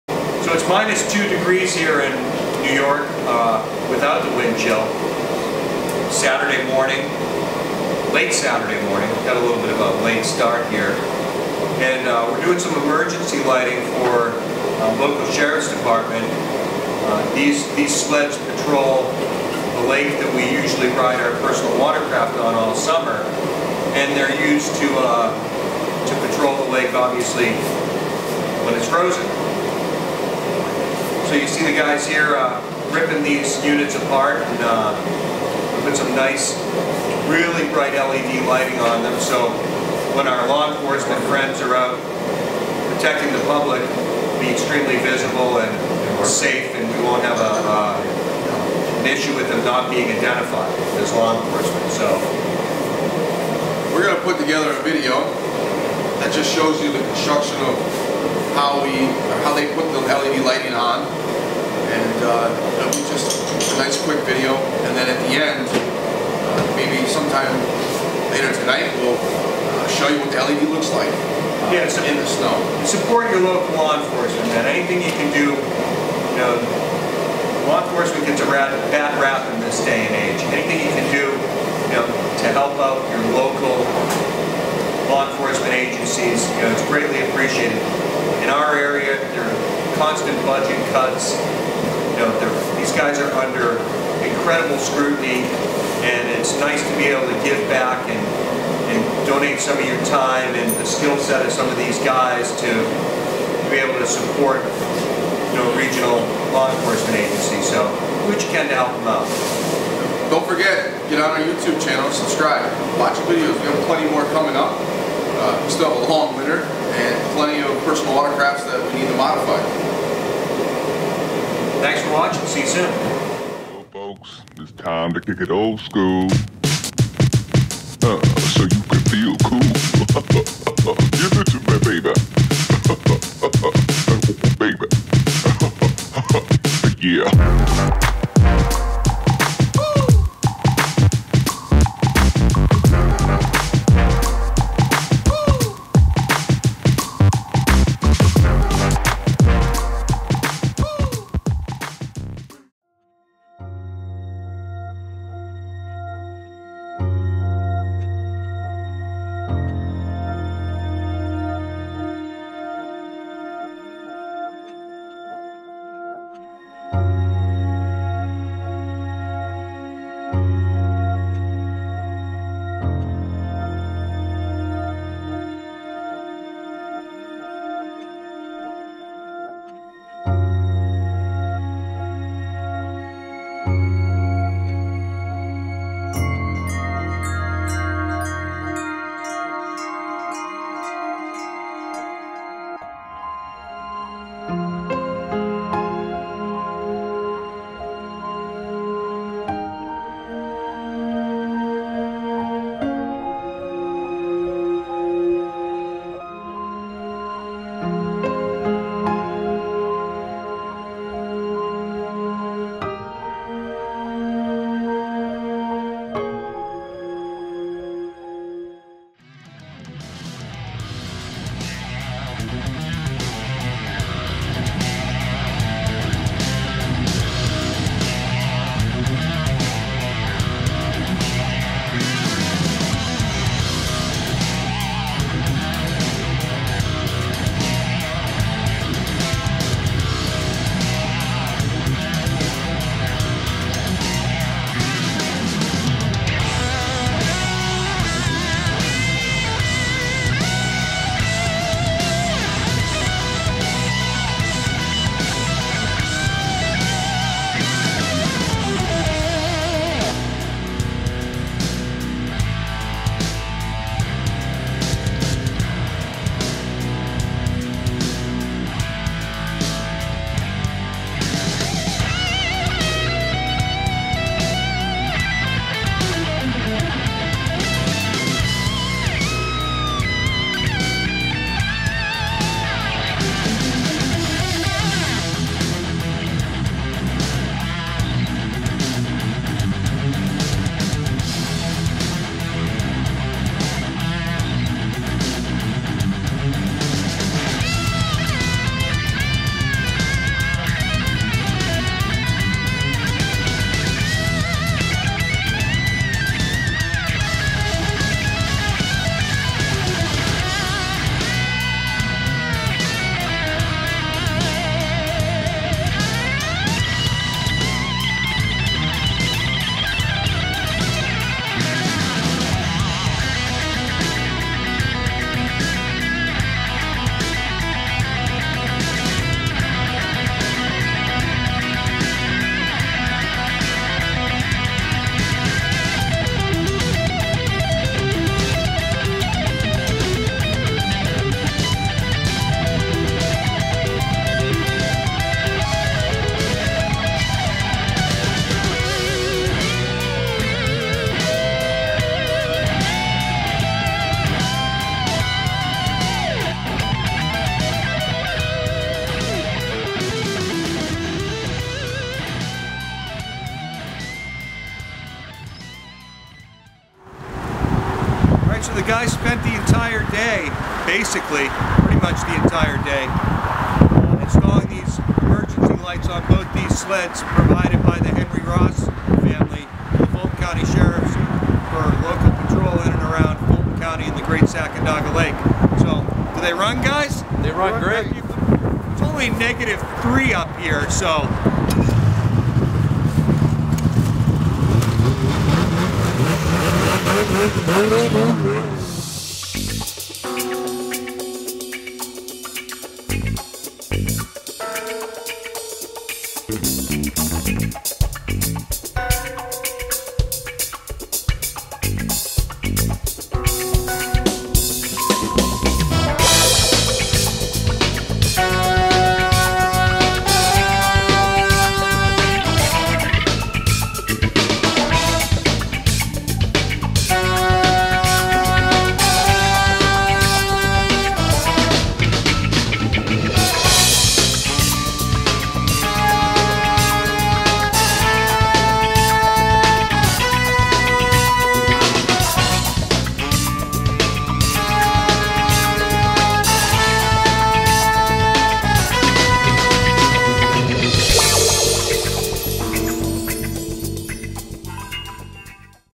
Emergency LED lighting for Sheriff Department
This video is about LED lighting. WaterCraft Network supporting law enforcement by donating shop time to install emergency lighting. We at Watercraft Network is committed to community service and supporting Police.